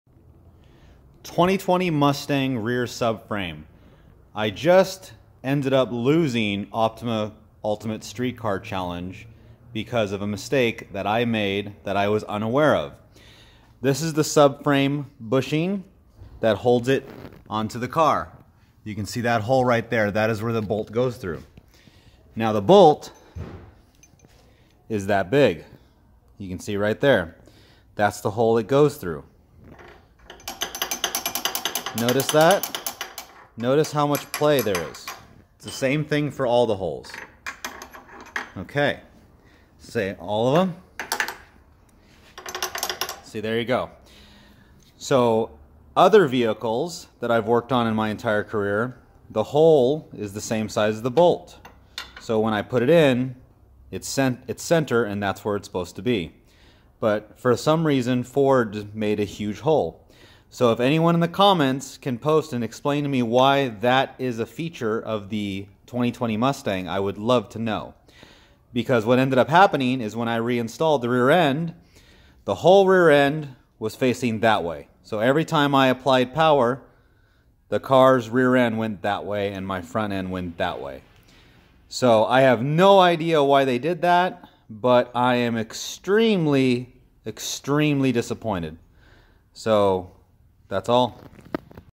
S550 Ford Mustang Rear Subframe Engineering Failure
someone please explain to me why the S550 mustang has massive bolt holes for rear subframe?